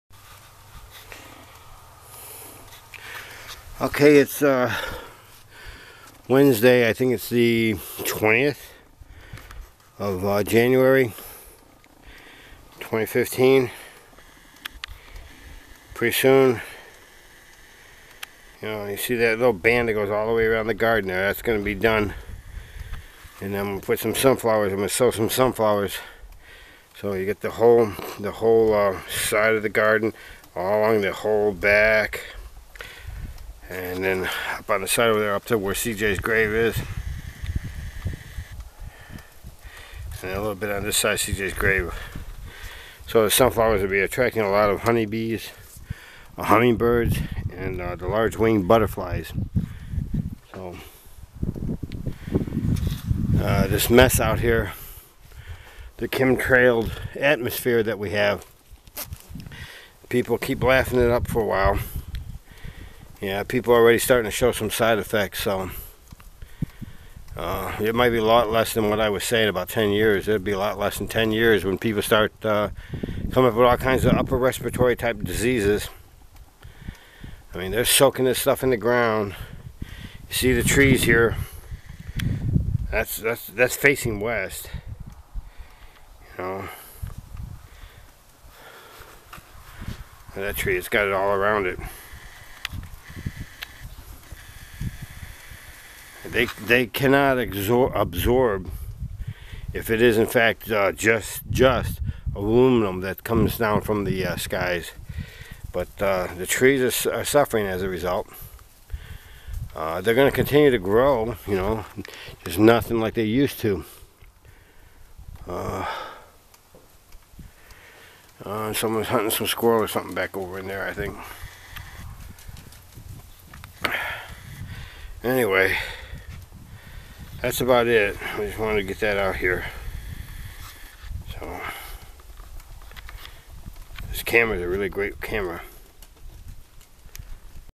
2016 01 20 Do WE Have Time
DO WE HAVE the Time? GLOBALISTS ARE destroying OUR planet. 99% DO NOT CARE ABOUT the GENERAL population, but TOO MANY "FALL for THEIR LIES"...STILL.